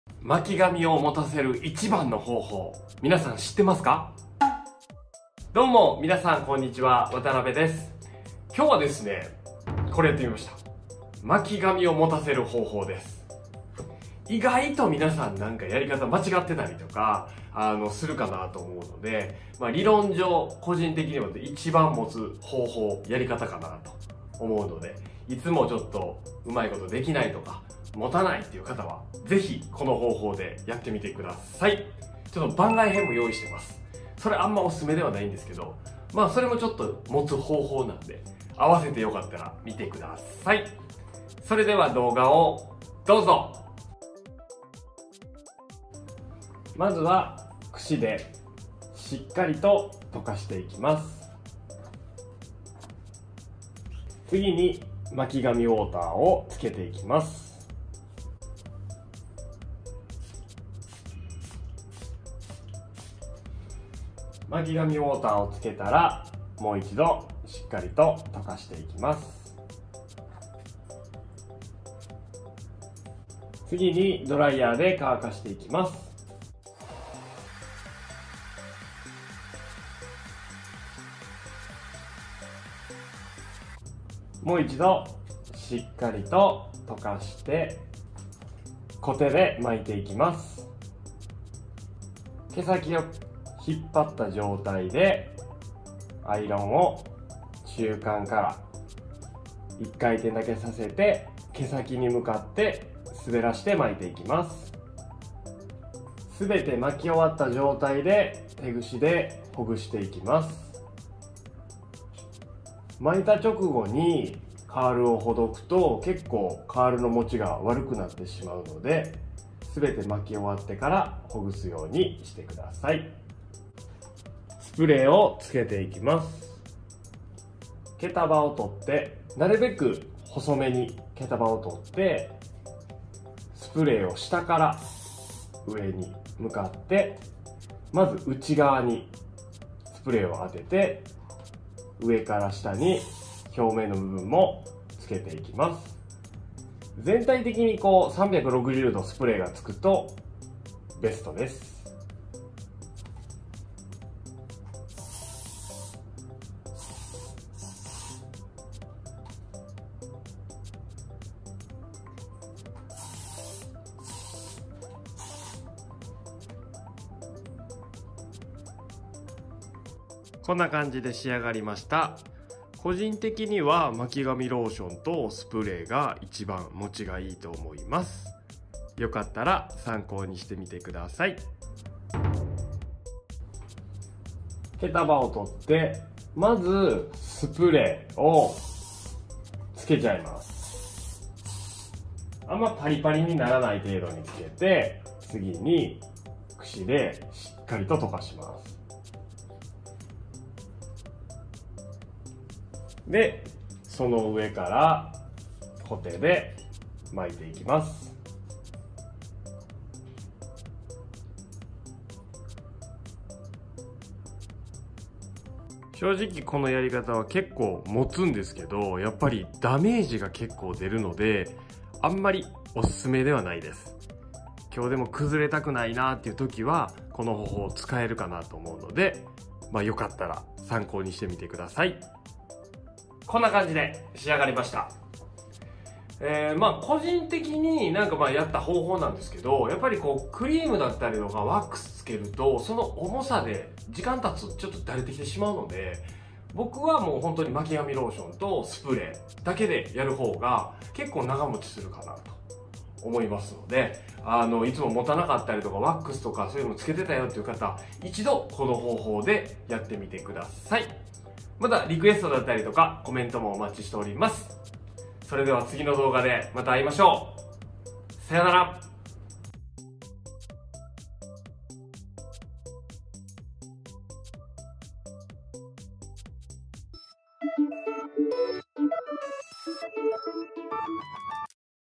今さら聞けない。巻き髪を長持ちさせる方法！！ クレイツ SALA ロレアル SALONTube 渡邊義明
巻き髪を保たせる基本な方法になります！

裏技もありますが、個人的にはあまりお勧めではありません笑

【使ったもの】
SALA巻き髪ウォーター
コーム
レプロナイザー

美容動画

Hair styling
Hair arrangement
头发
헤어